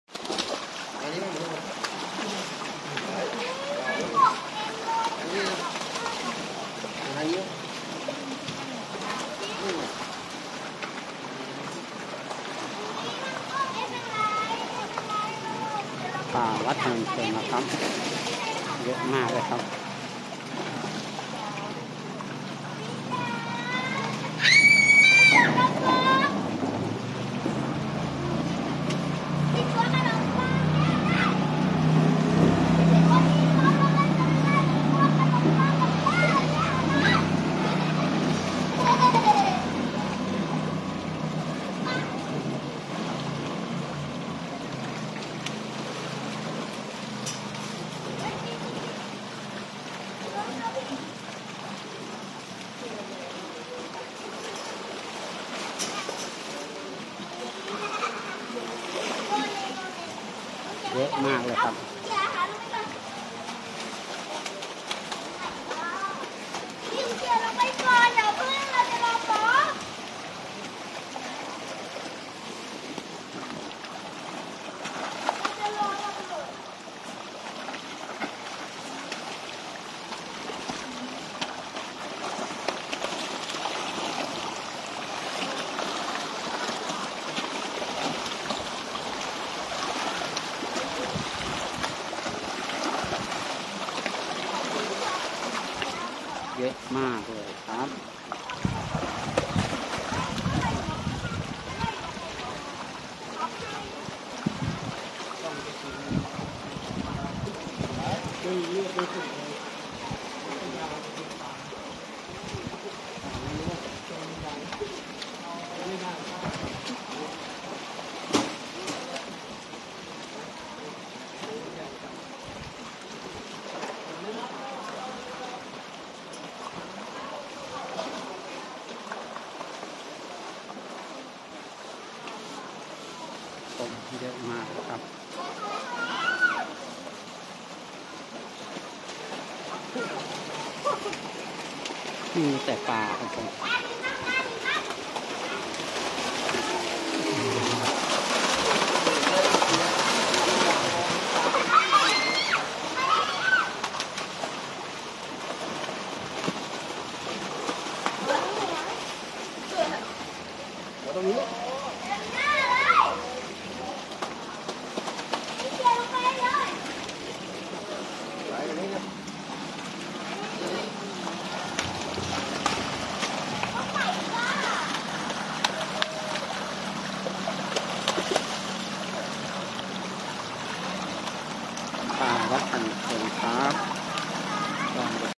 วัดพนัญเชิงEP11
มาให้อาหารปลาที่วัดพนัญเชิงจังหวัดอยุธยาครับ